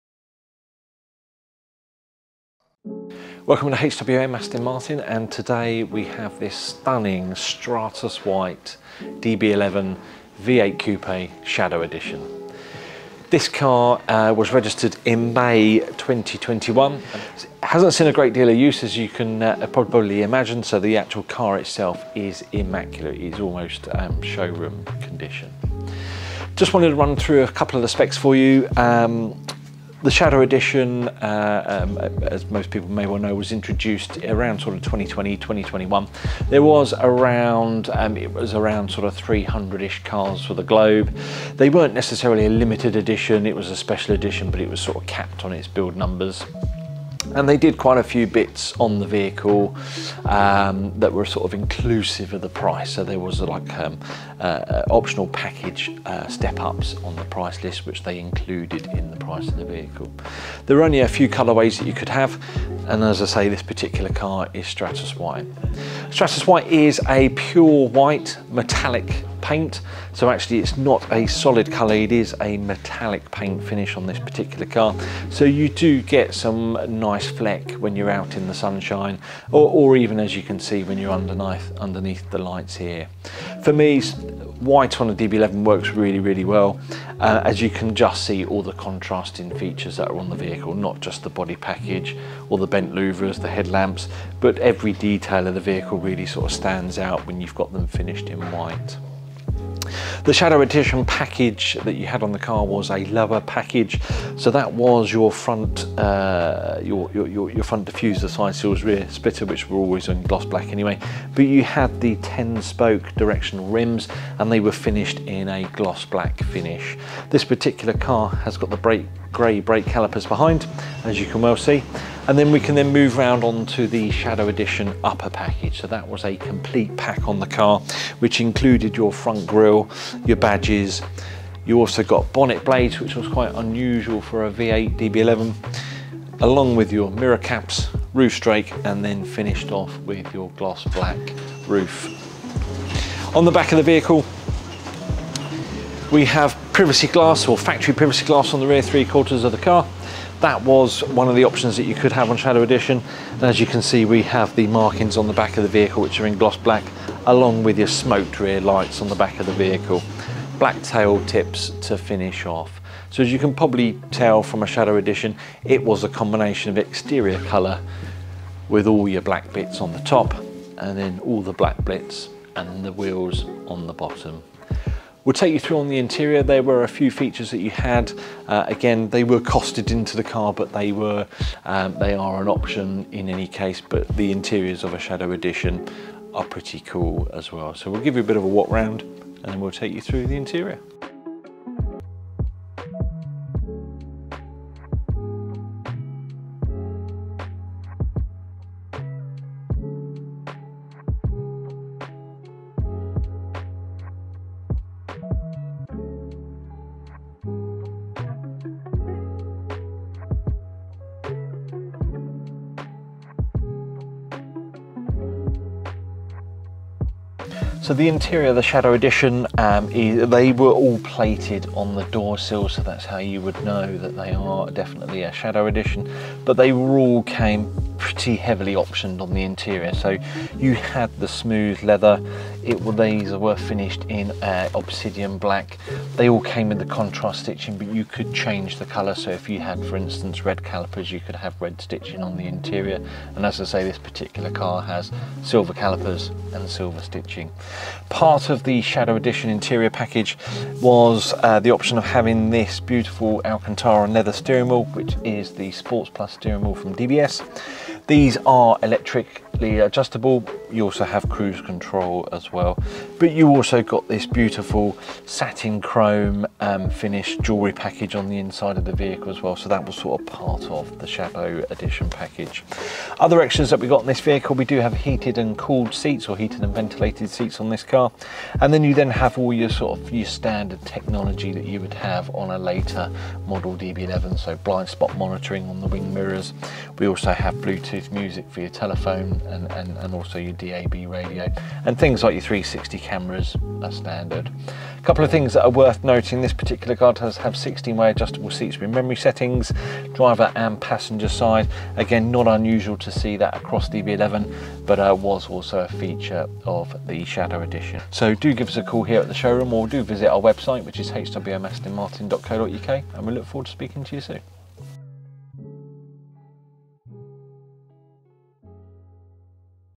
A Timeless Aston Martin DB11 Shadow Edition Finished in Stratus White - A Walk Around With Stuart
The Aston Martin DB11 Shadow Edition was limited to just 300 units globally and was available in both Coupe and Volante variants. HWM Aston Martin, the worlds longest serving Aston Martin dealership are delighted to offer this DB11 V8 4.0 Coupe Shadow Edition which has covered only 650 miles from new. Upgrades on the Shadow Edition include gloss black bonnet blades, 20-inch gloss black directional alloy wheels, unique black Shadow Edition sill plaques, and polished chrome wings and script badges. Other Spec highlights include Heated and Ventilated Seats, Aston Martin Premium Audio, Smoked Rear Lights and the all important Technology Package, to name just a few. The GT Sports Steering Wheel is wrapped in Pure Black Alcantara and Obsidian Black Leather, an upgrade on the standard wheel. Marek Reichman, Chief Creative Officer, Aston Martin Lagonda stated "Aston Martin have carefully selected the Shadow Edition design elements to subtly yet noticeably alter the DB11's appearance – moving it further towards a more assertive and purposeful GT style," Stunning from just about every angle this high spec Shadow Edition DB11 can be reserved quickly and securely for just £99